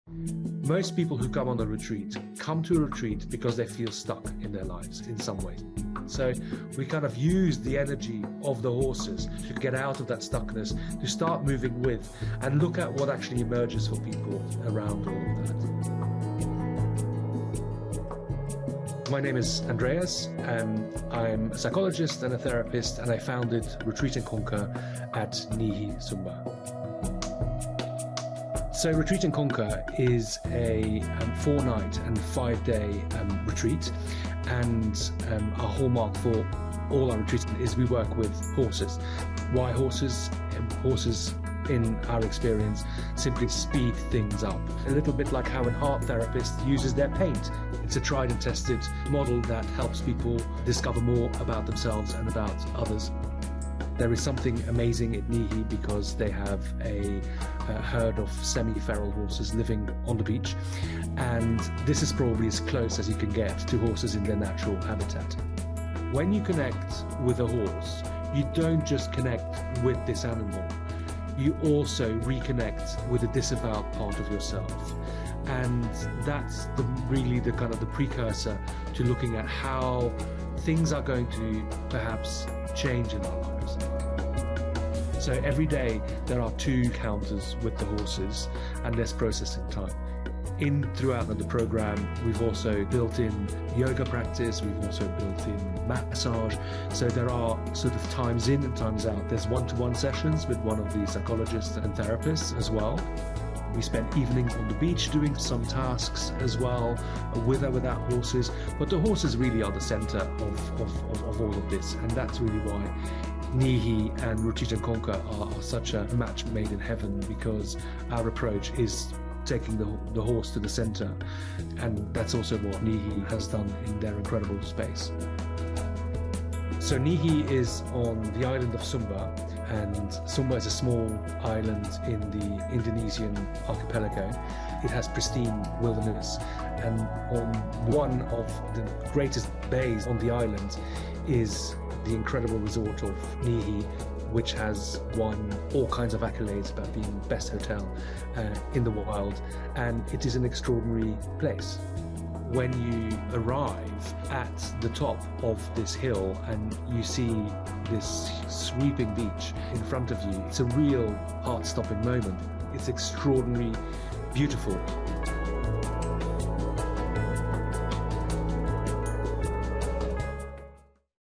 What is equine therapy? This paradise island promises healing through horses
‘Horses, in our experience, simply speed things up, a little bit like how an art therapist uses their paint.’